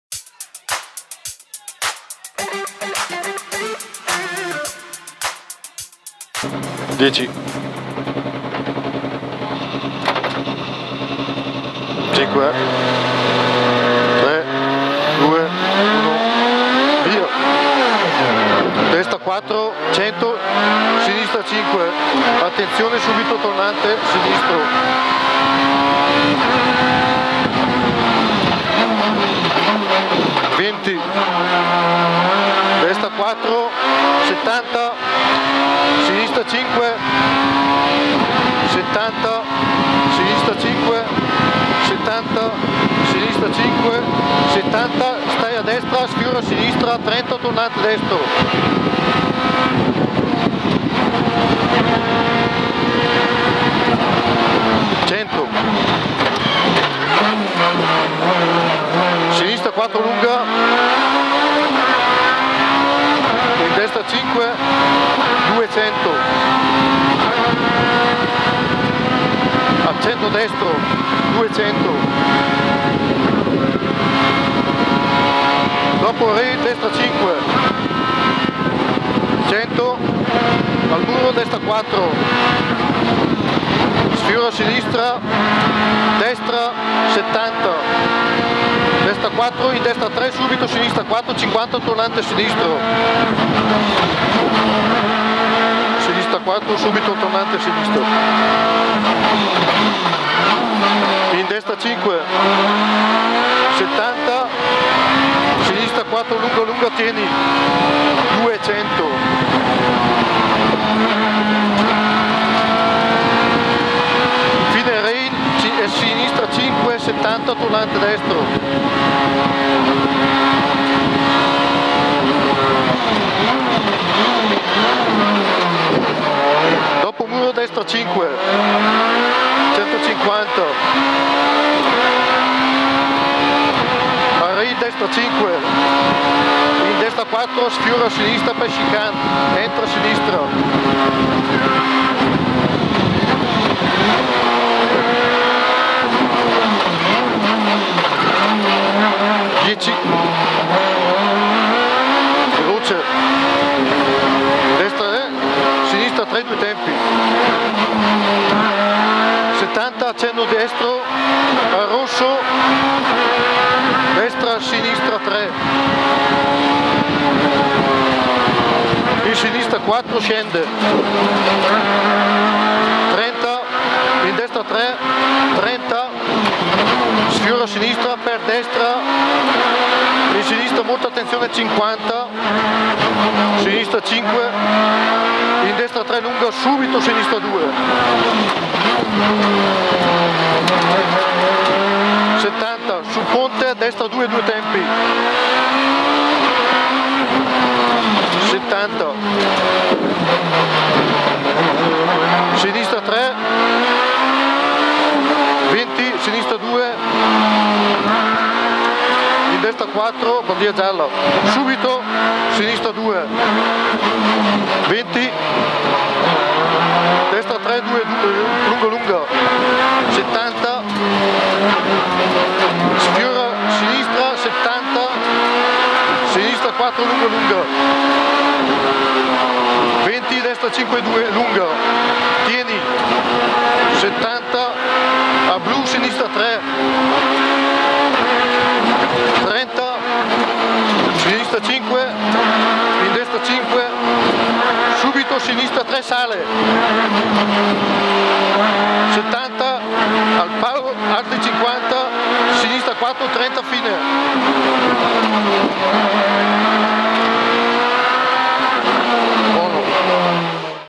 OBC 9°Rally del Sebino 2021 TRAPPA-PIARDI by Ferrario
Onboard con l'equipaggio TRAPPA-PIARDI su Renault Clio S1600 durante la ps 4 del 9°Rally del Sebino 2021.